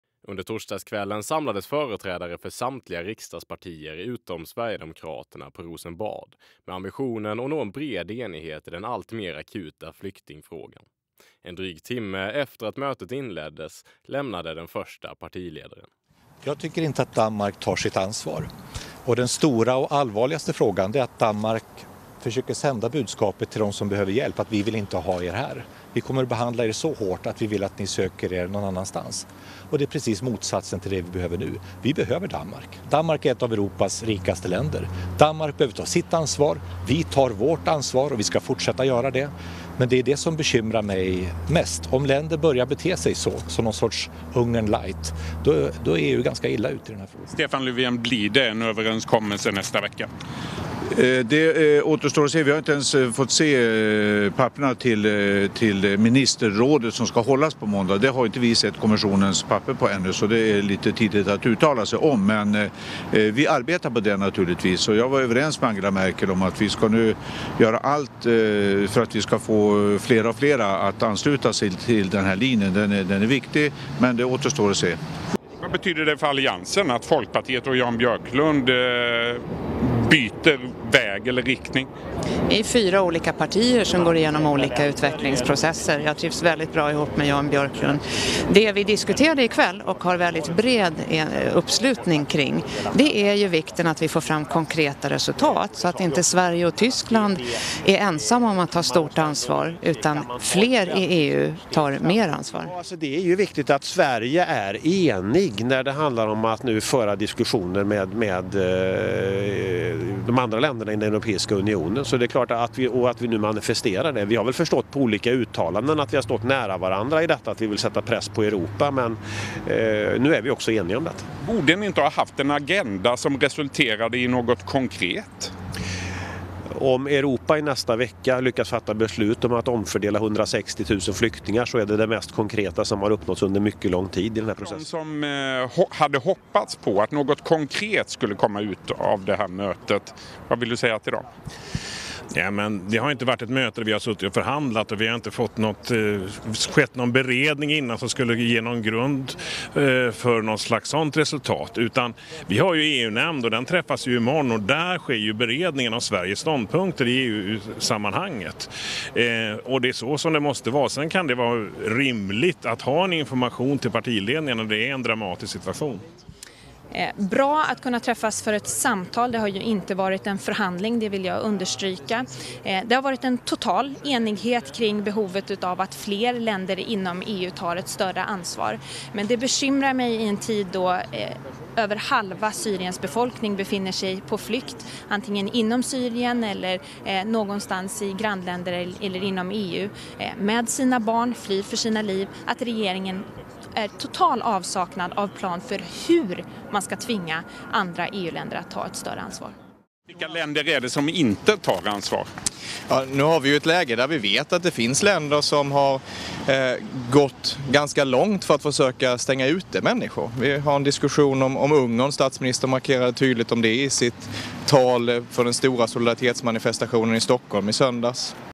Löfven: "Vi ska ha ordning och reda"
Partiledarna lämnar Rosenbad efter regeringens sammanträde